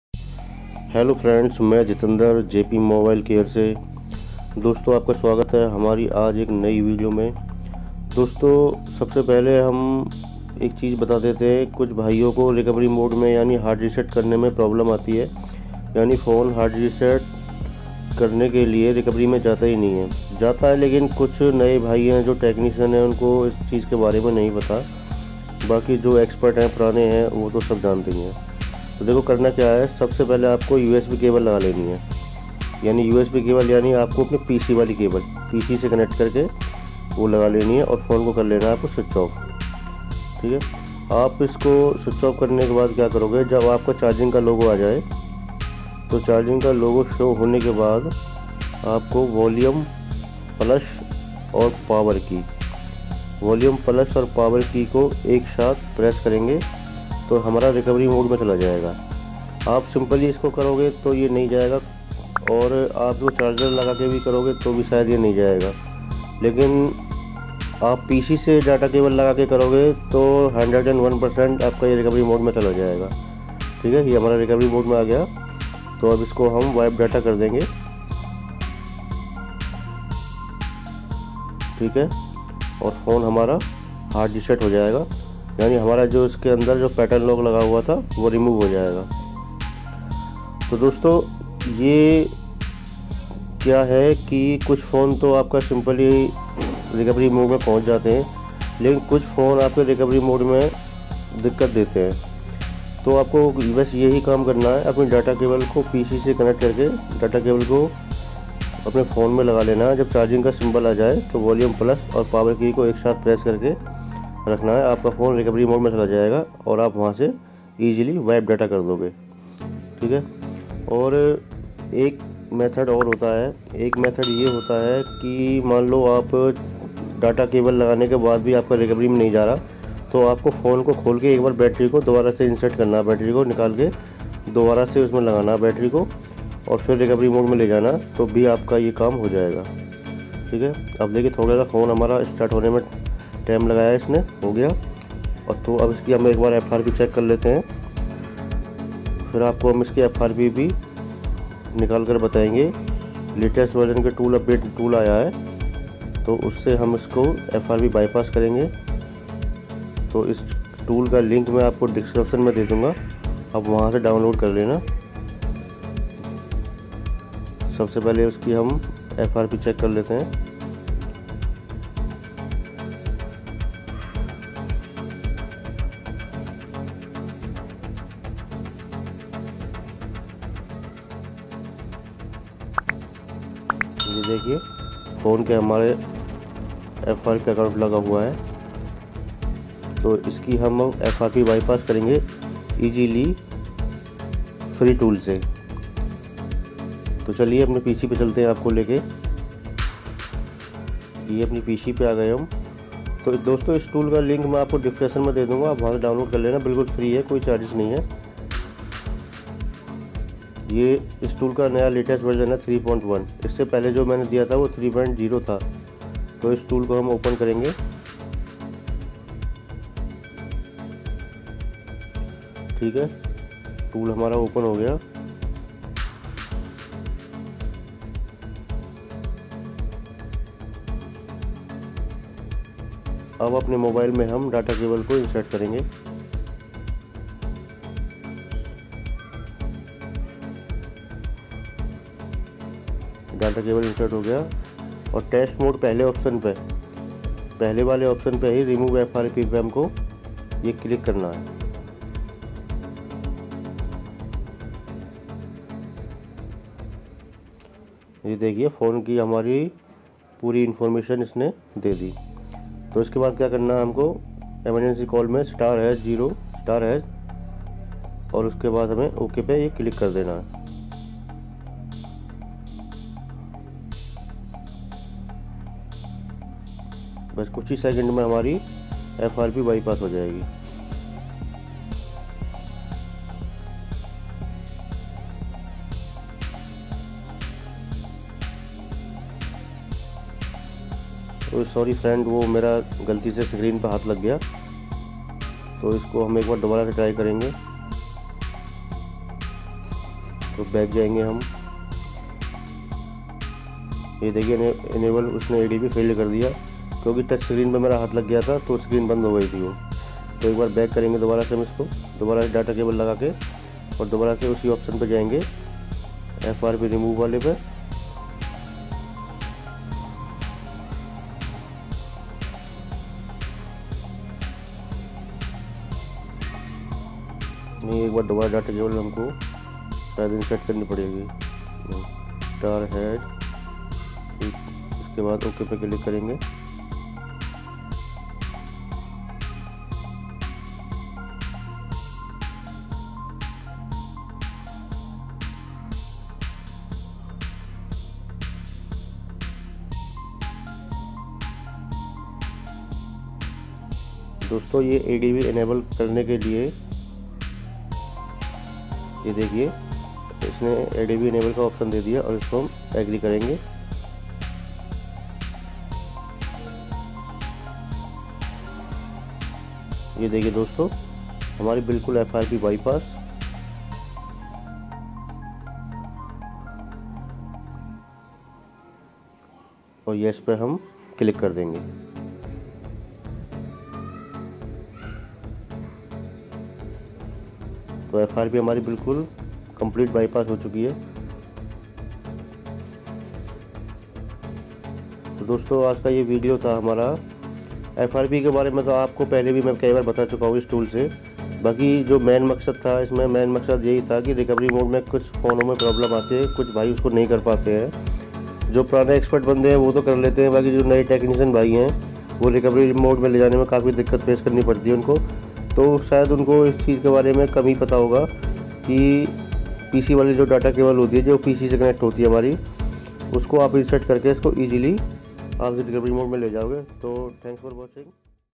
All Samsung Frp Bypass Free Tool/Hard reset recovery mode fix!!
How to bypass FRP lock on Samsung with PC